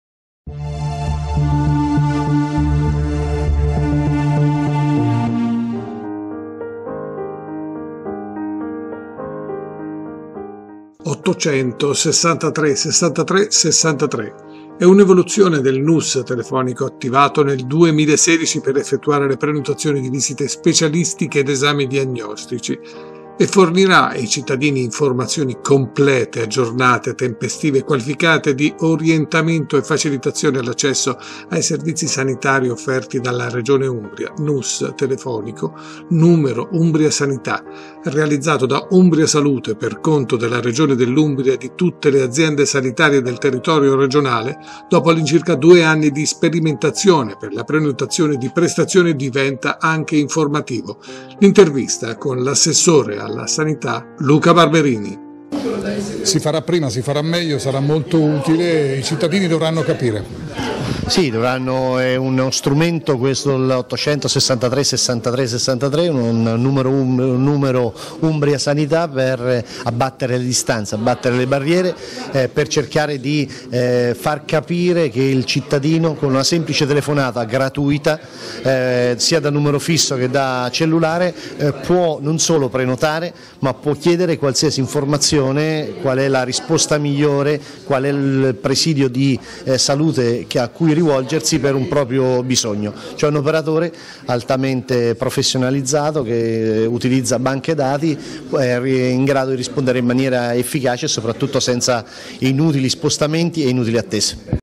800 63 63 63 NUS Numero unico sanità in Umbria per Cup e prenotazioni 01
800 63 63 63 NUS Numero unico sanità in Umbria per Cup e prenotazioni --

Numero Umbria sanità, NUS, che ridurrà anche le code, è il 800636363 E’ un’evoluzione del NUS telefonico attivato nel 2016 per effettuare le prenotazioni di visite specialistiche e esami diagnostici, e fornirà ai cittadini informazioni complete, aggiornate, tempestive e qualificate di orientamento e facilitazione all’accesso ai servizi sanitari offerti dalla Regione Umbria: il NUS telefonico (Numero Umbria Sanità), realizzato da Umbria Salute, per conto della Regione Umbria e di tutte le Aziende Sanitarie del territorio regionale, dopo all’incirca 2 anni di sperimentazione per la prenotazione di prestazioni, diventa anche informativo.

Leggi anche: Ecco il contact center telefonico

Il servizio è stato presentato stamani a Perugia nel corso di una conferenza stampa alla quale ha partecipato l’assessore regionale alla Salute, Coesione sociale e Welfare, Luca Barberini, insieme all’amministratore unico di Umbria Salute, Piero Rosi, e Mara Sforna, direttore dei Servizi all’utenza di Umbria Salute. Per l’occasione è stata illustrata anche la campagna regionale di comunicazione per promuovere il nuovo servizio.

Il NUS risponde al numero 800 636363